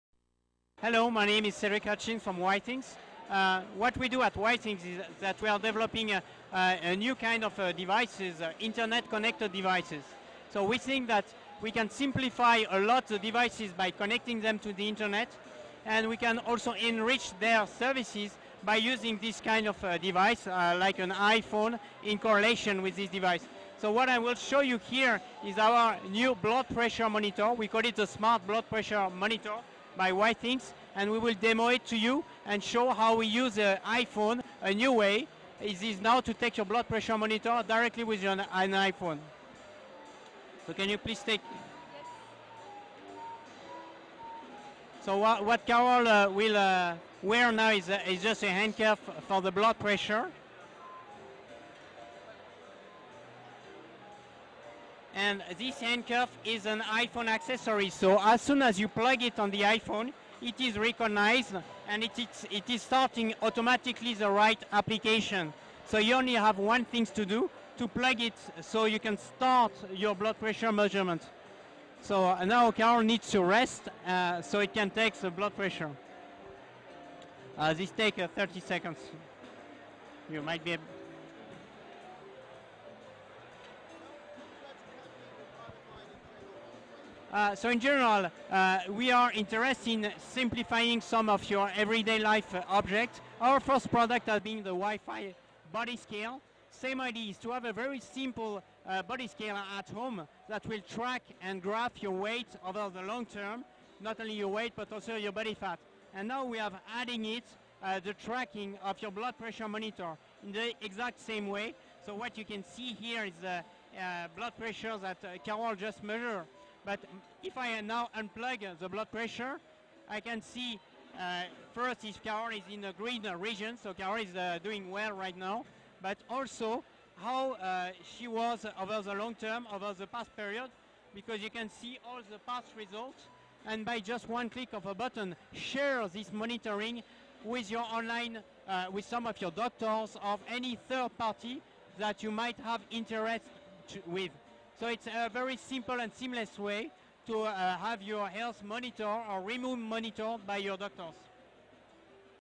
Review: Blood Pressure Reader App by Withings at Consumer Electronics Show 2011
E27.sg app review on Withings blood pressure reader for the iOS at Consumer ELectronic

Designed specifically for IOS devices, it instantly provides a precise measurement of your blood pressure.